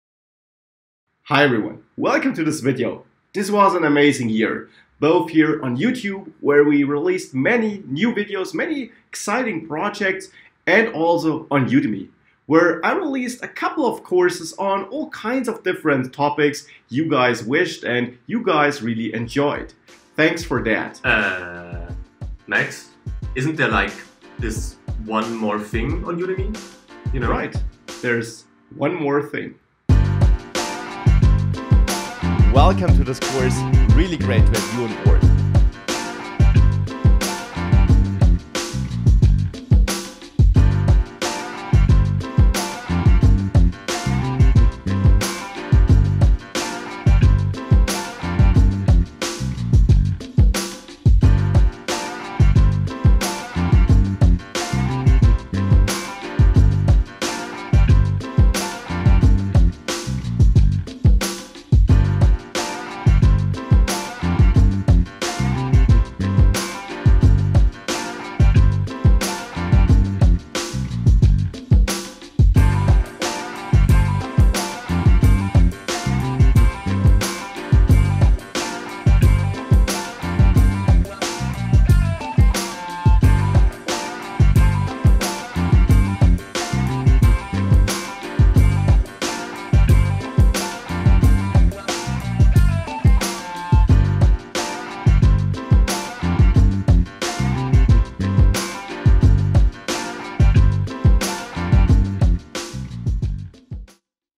React Native | New Course | OUT NOW!
One more thing in 2017 - Here it is, my brand new Udemy course "React Native - The Practical Guide". Join now and learn how to use your React knowledge to creative awesome Native apps for iOS and Android!

• You can follow Max on Twitter (@maxedapps).